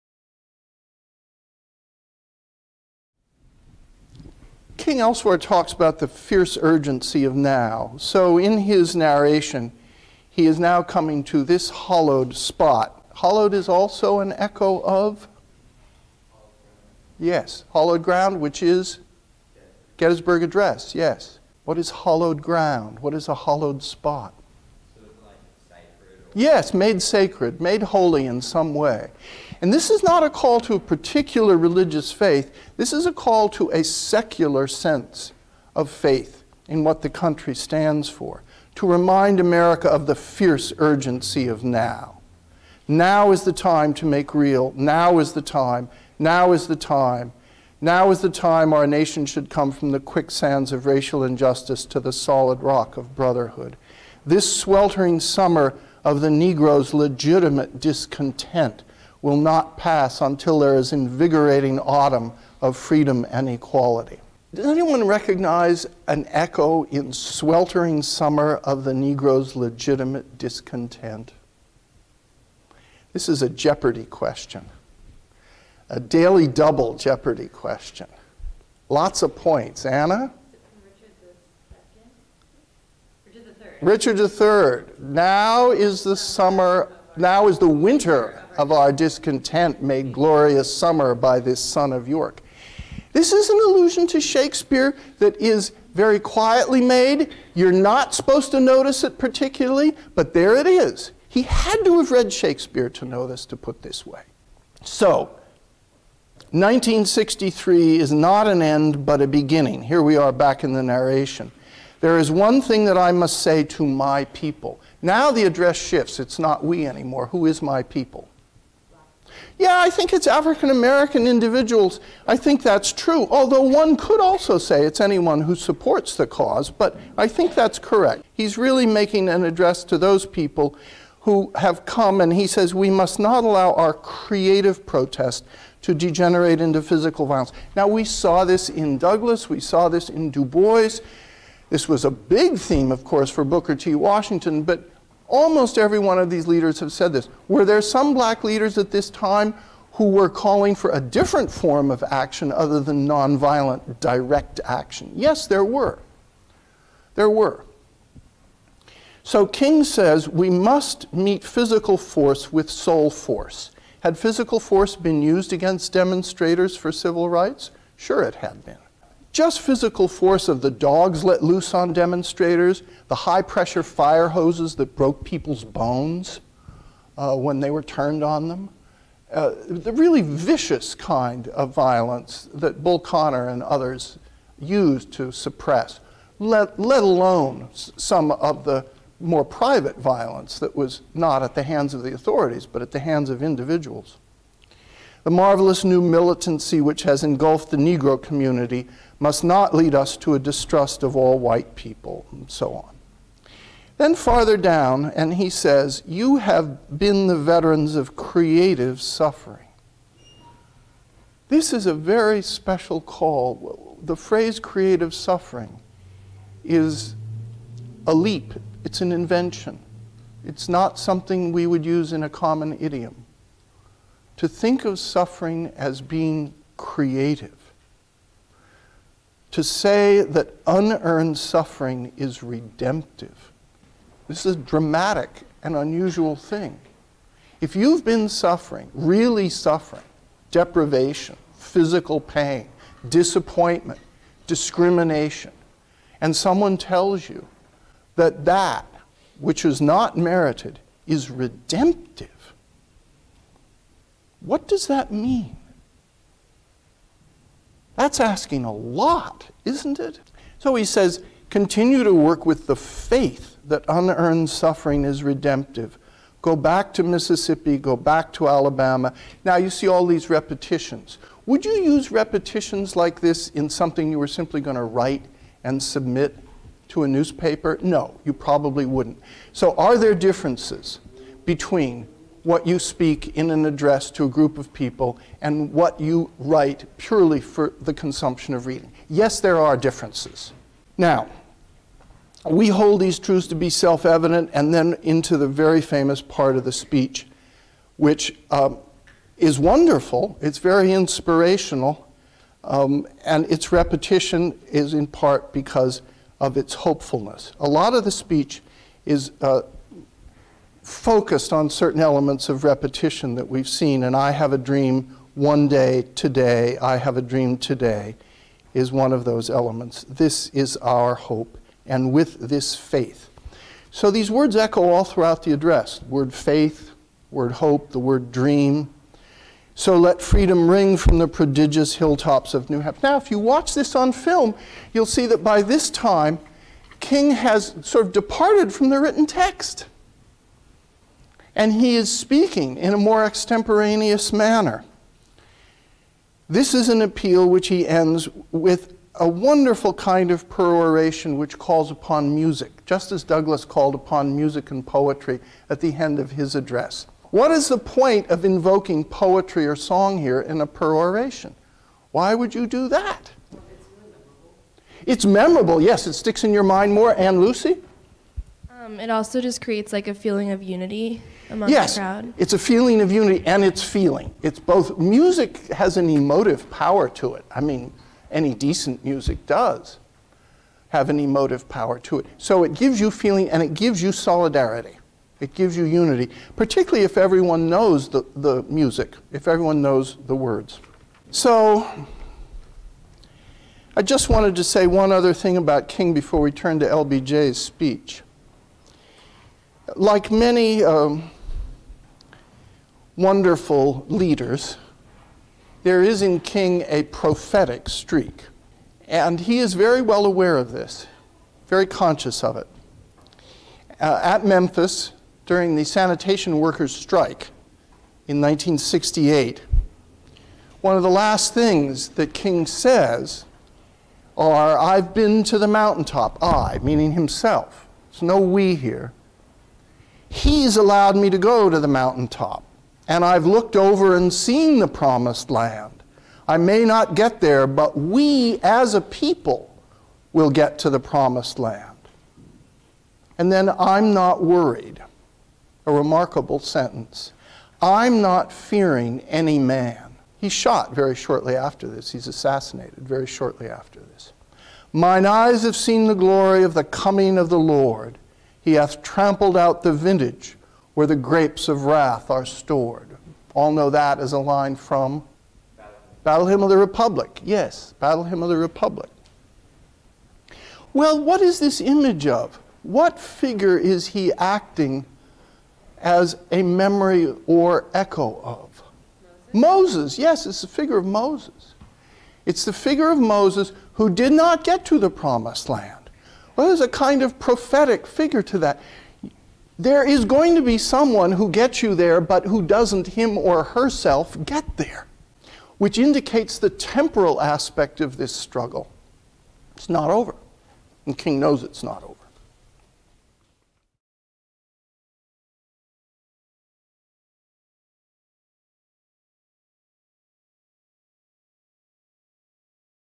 Martin Luther King, Jr.'s "I Have a Dream" Part: 2 |The Art of Persuassive Writing & Public Speaking
Professor Engell points to the importance of setting to the crafting of a speech. He also describes Martin Luther King Jr.'s use of allusion and metaphor as instruments of oratory.